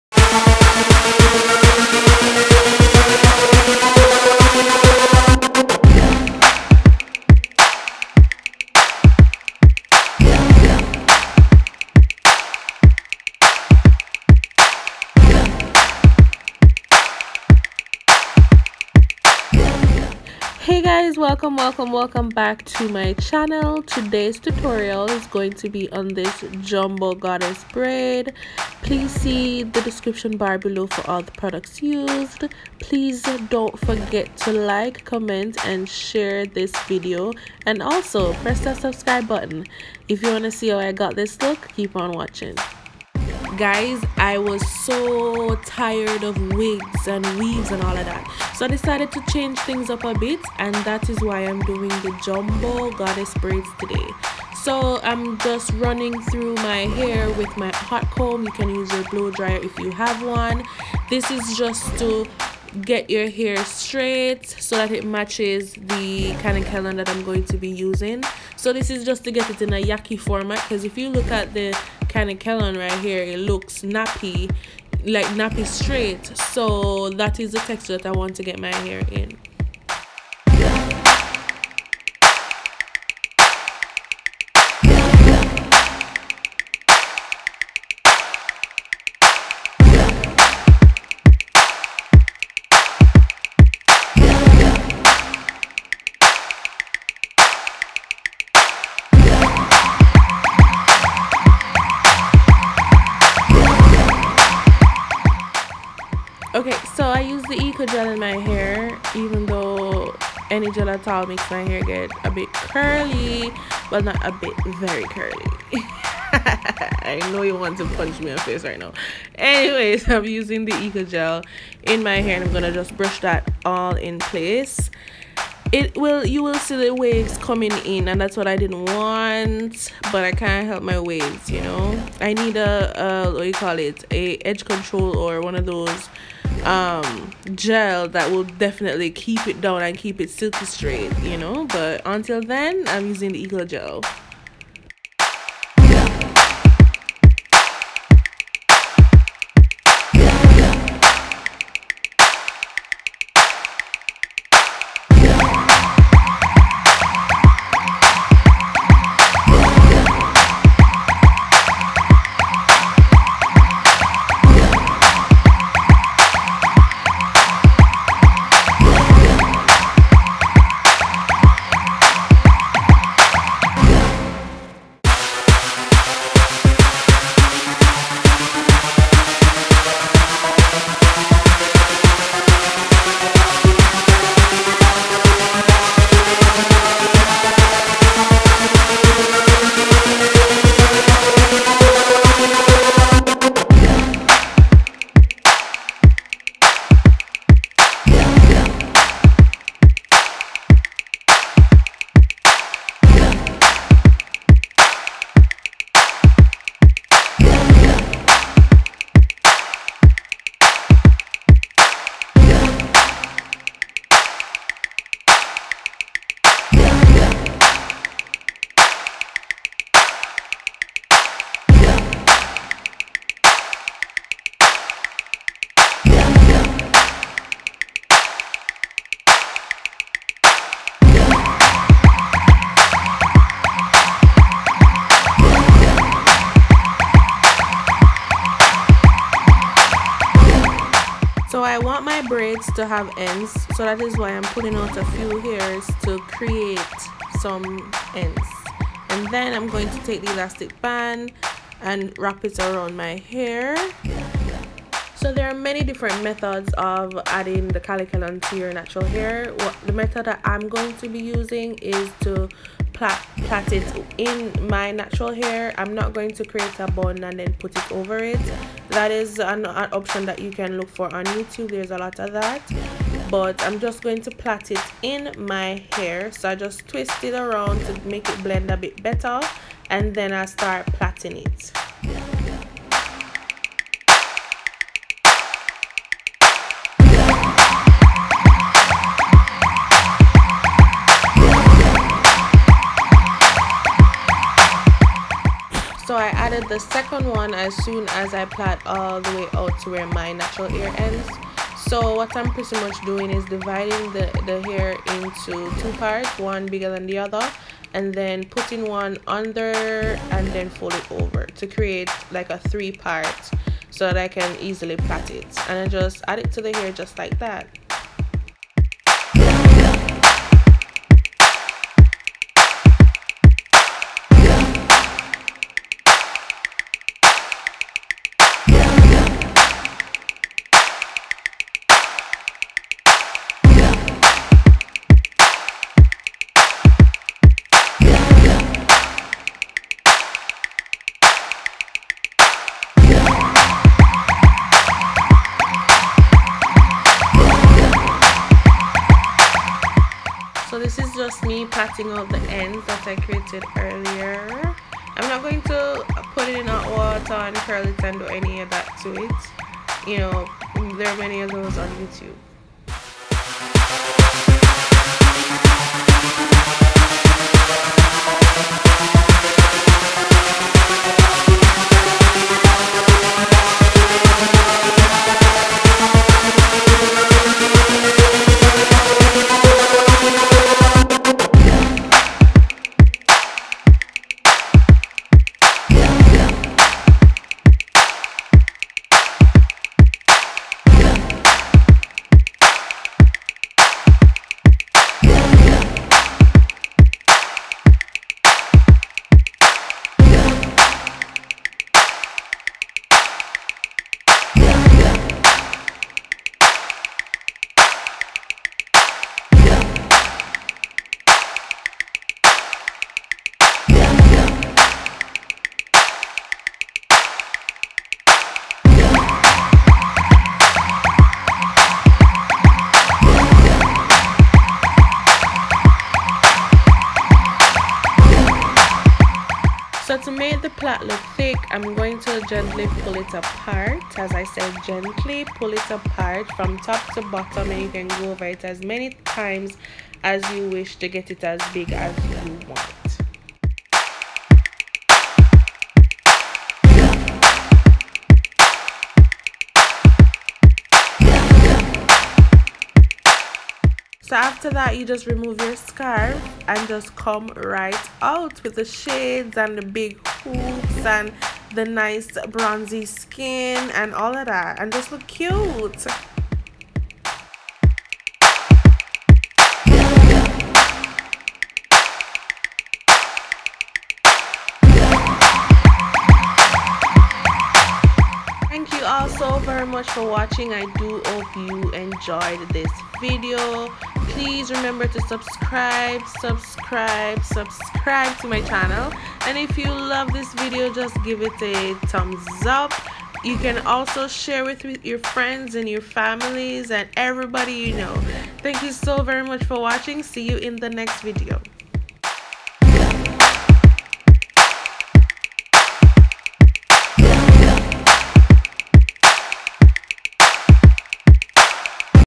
How to: JUMBO BRAID | kanekalon | Natural Hair
This video is a tutorial on how I did the Jumbo Braid using the materials below. If you have any questions please ask them in the comment section below.

Materials Used
1. Que volume 3x Jumbo braid – style King jumbo Braid – colour 1B
2. Eco Style Professional styling gel
3. Hair ties and hair pins
4. Tooth brush
5. Hair brush
6. Scarf

Video information
For recording -  IPhone 6s plus
For editing - IMovie
For lighting - Neewer ring light